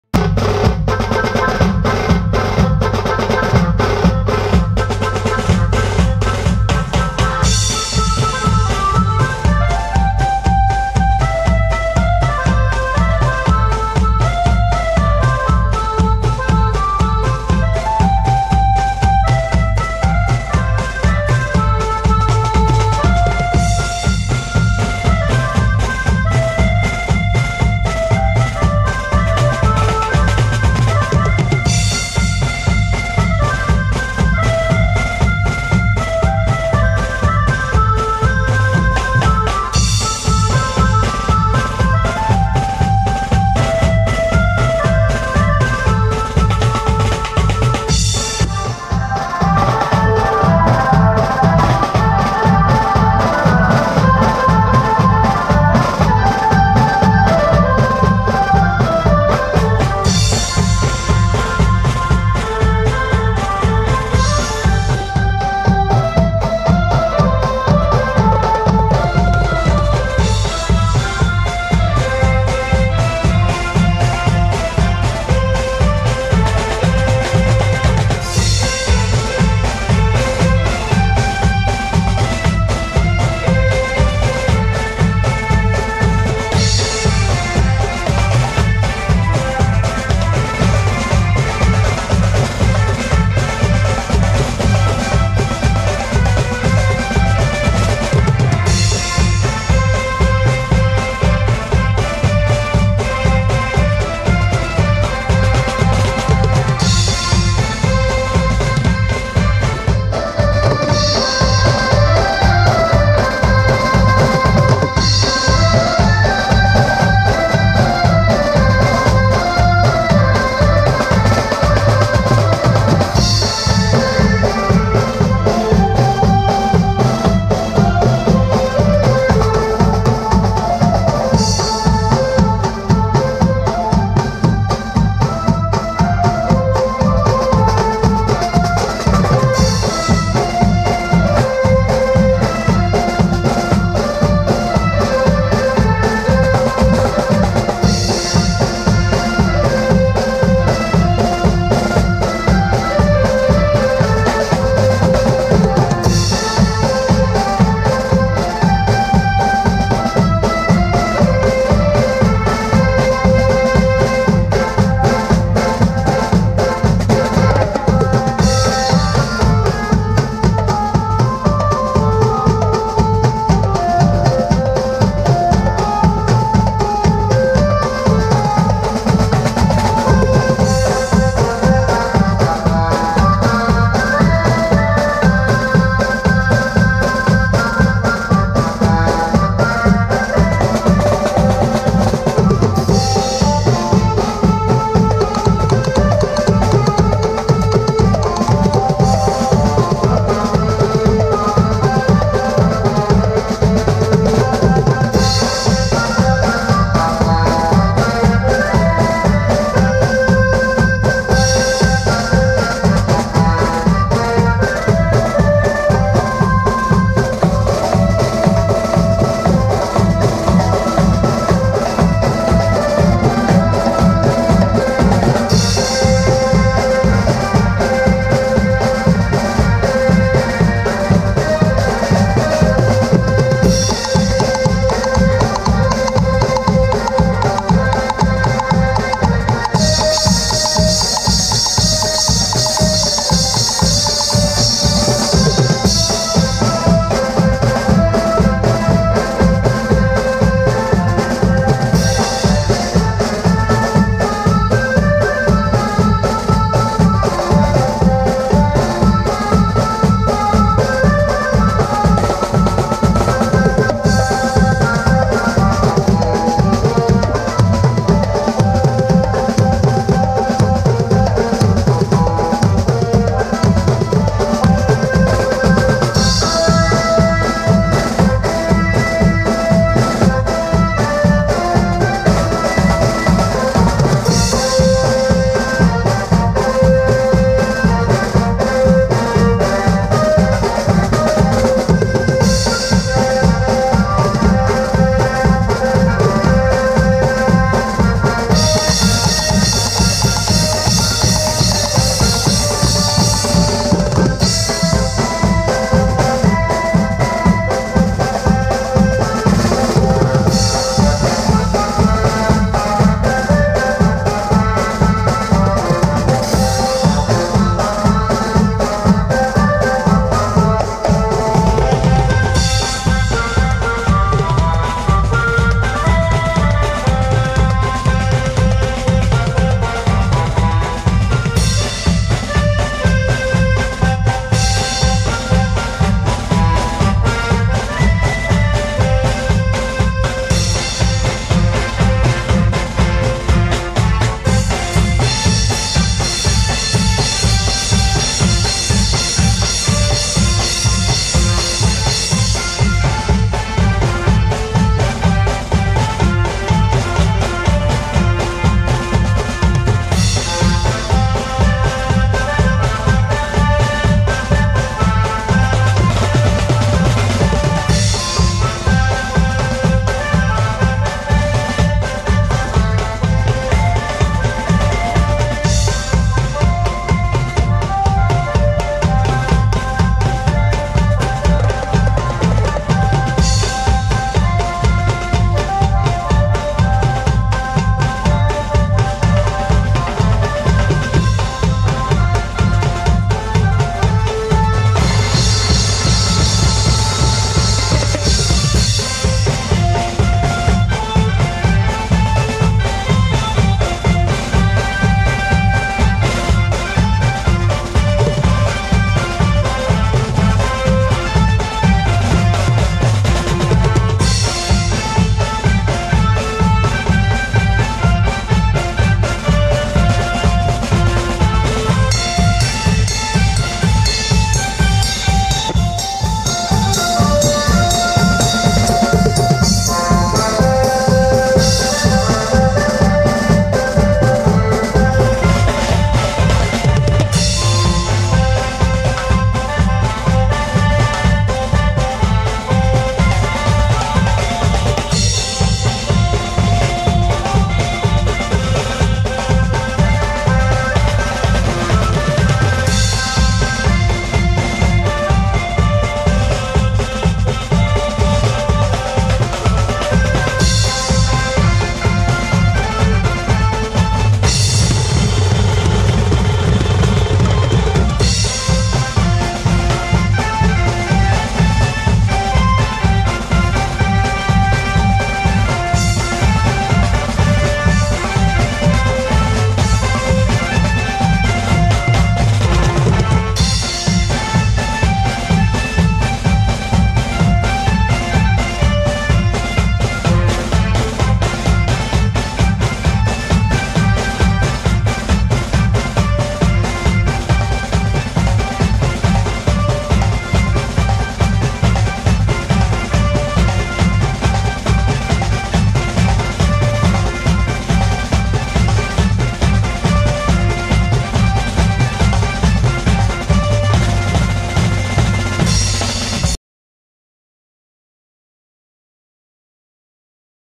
Sarang Beats🔥 || Chatrapti Shivaji Maharaj Old Songs - Shivjayanti, Turbhe 2020 || Musical Group
Sarang Beats (Malad)

Shivjayanti 2020, Turbhe

Navi Mumbai, Maharashtra

Chatrapati Shivaji Maharaj Old Songs

Marathi Old Songs